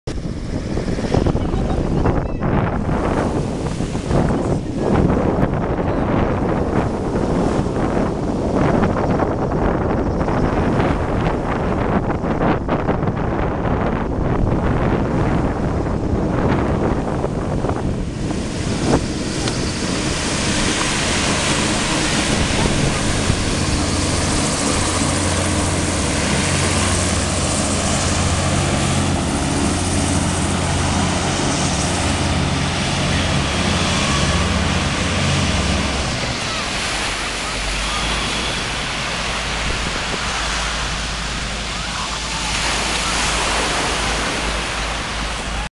Hovertravel hovercraft arrives in Portsmouth.
The Hovertravel hovercraft with the excellent Union jack/flag arrives in Portsmouth.